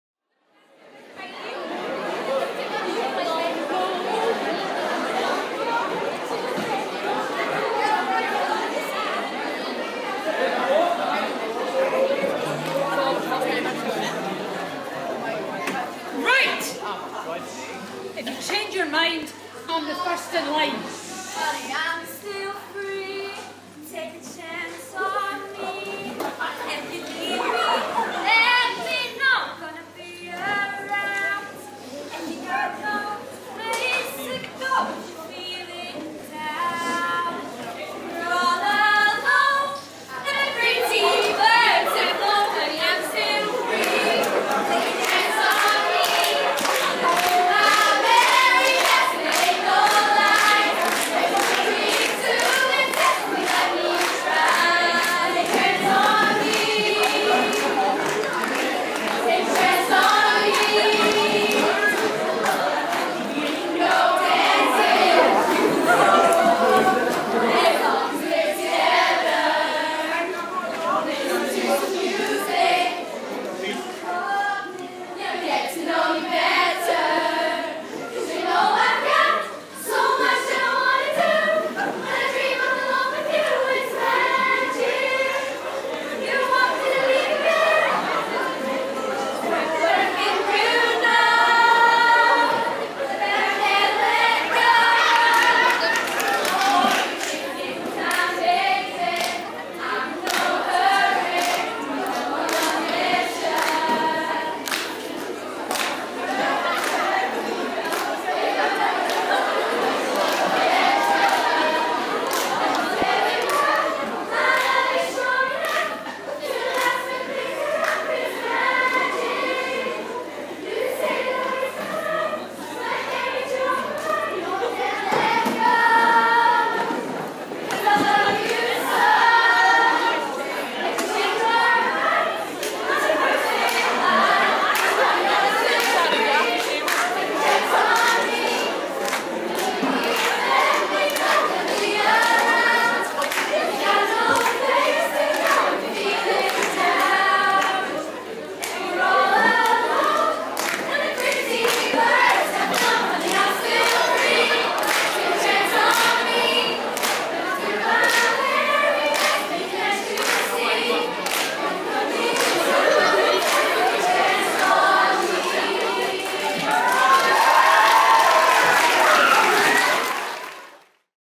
Cardinal Newman High School & the National Youth Film Fetival 2013 - Flashmob
Cardinal Newman School in Scotland performed this brilliant Flashmob as part of the new National Youth Film Festival 2013 Celebrations.

They were asked to pick a scene from their favourite filim and perform it live...and this is what they did!

We're so impressed with all of the hard work and practice that has gone in to make this happen.

Well done Cardinal Newman School, thank you for being such an fun and exciting part of our celebrations.